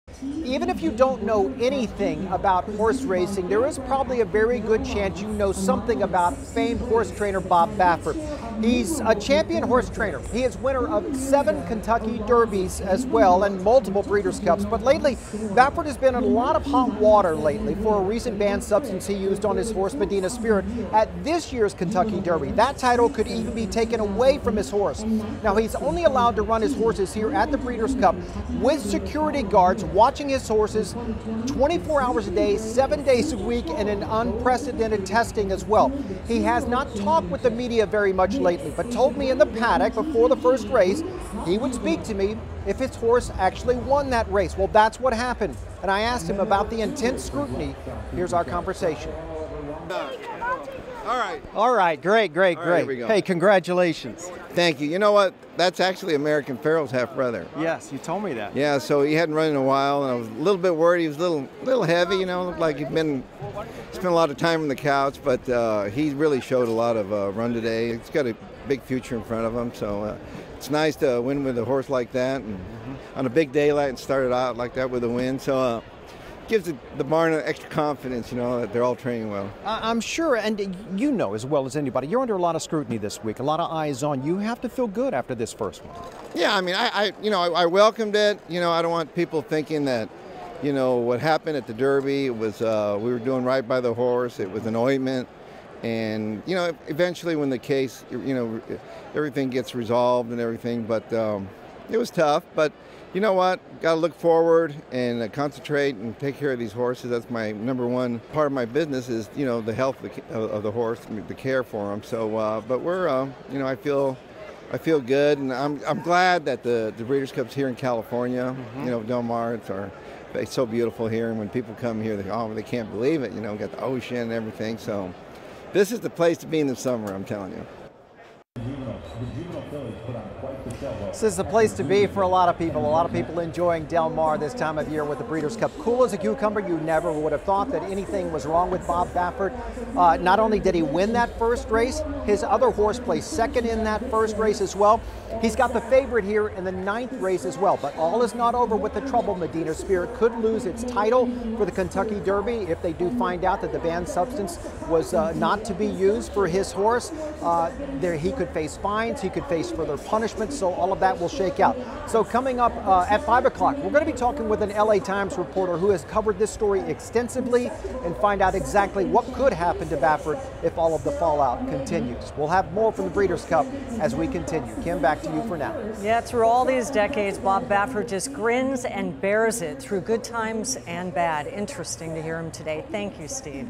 One-on-one with horse trainer Bob Baffert at Breeders' Cup in Del Mar
Horse trainer Bob Baffert spoke with ABC 10News anchor Steve Atkinson at Del Mar after his horse won the first race in the Breeders' Cup series on Friday.